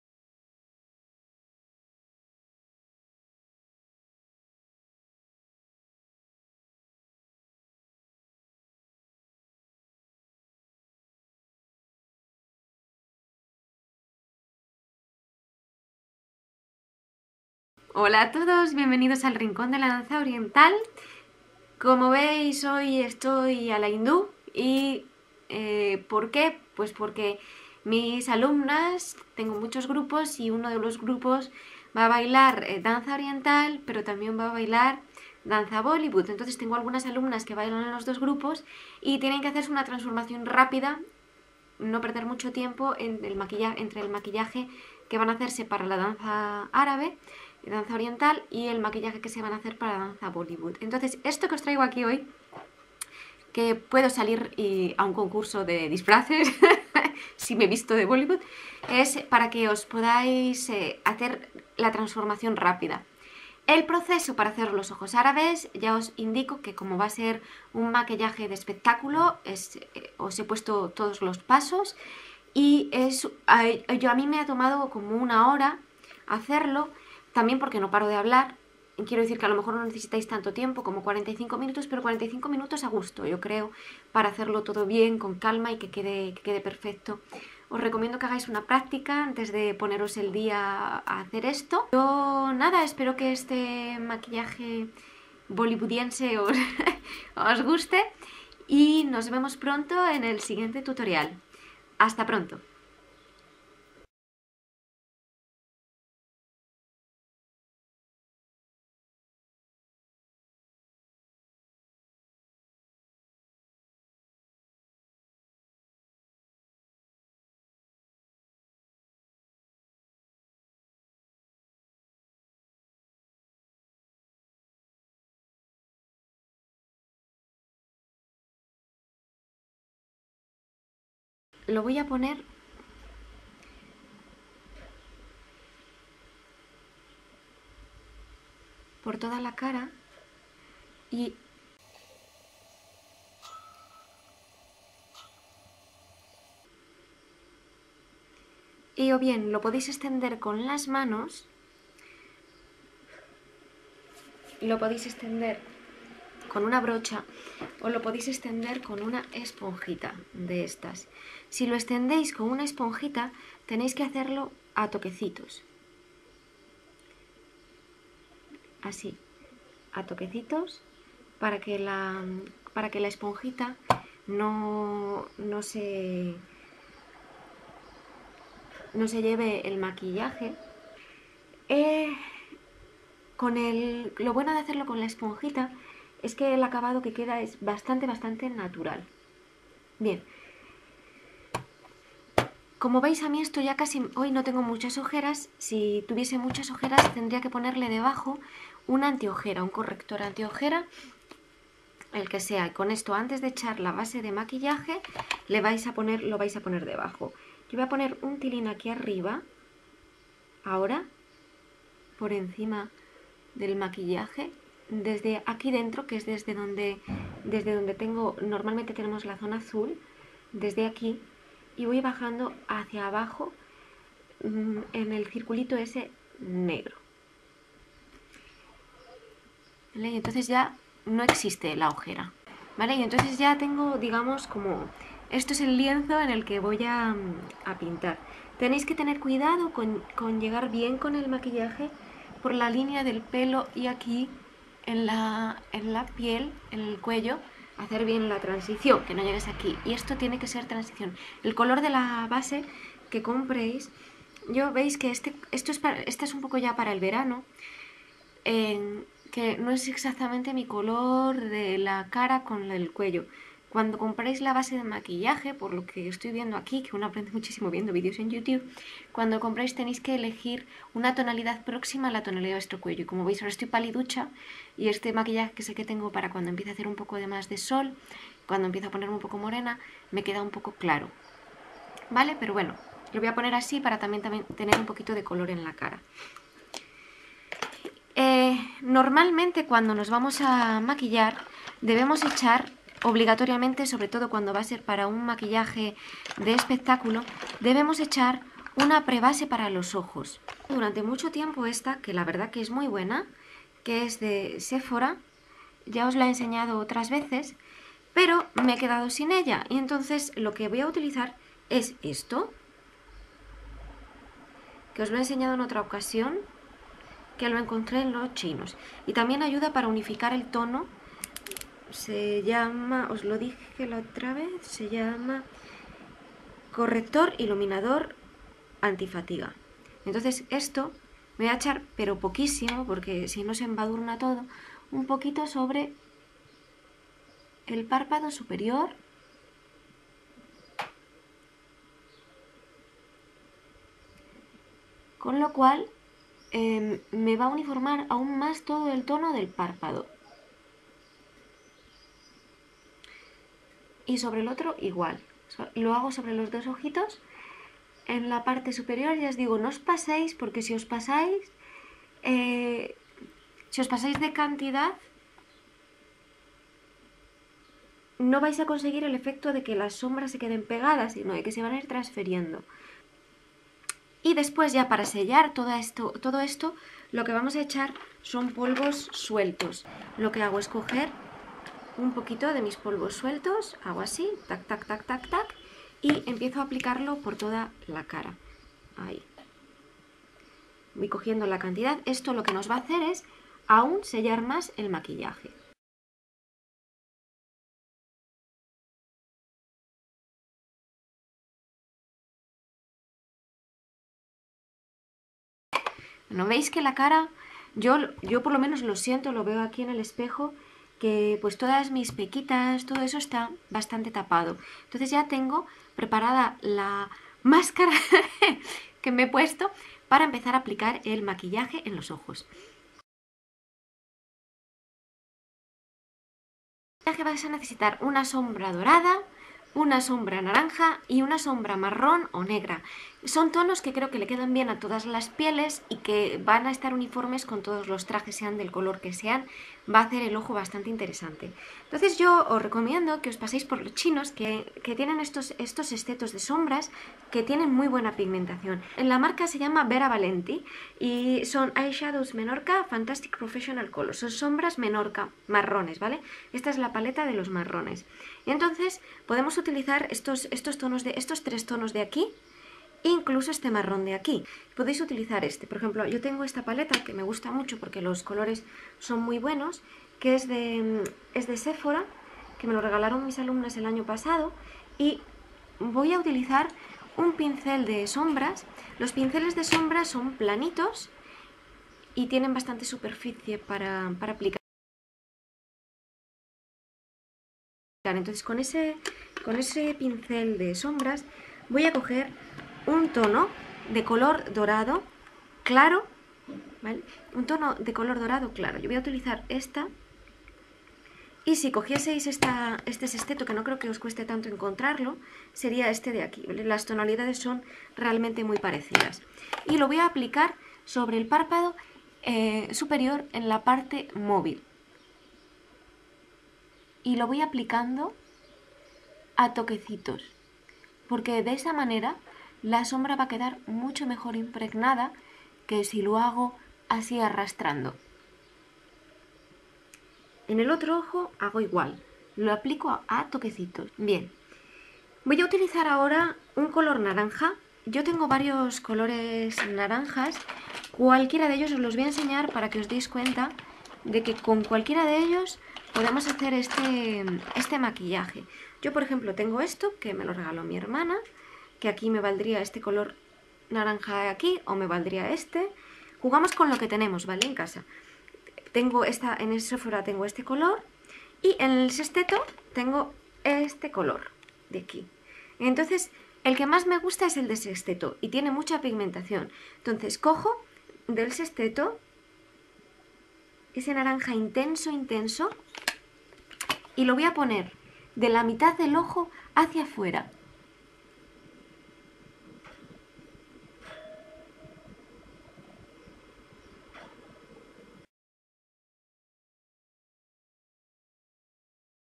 Maquillaje de escena. Danza oriental a bollywood (parte 1) / Show make up Bellydance to Bollywood
Con esta serie de vídeos quiero enseñaros a transformar un maquillaje de danza oriental a bollywood. Son cuatro vídeos, porque he querido mostraros detalladamente el proceso completo.  ¡¡Llevo días editando!! Jajaja
Espero que os guste el resultado.

En esta primera parte veremos cómo preparar el rostro y empezaremos con la primera versión del maquillaje de los ojos.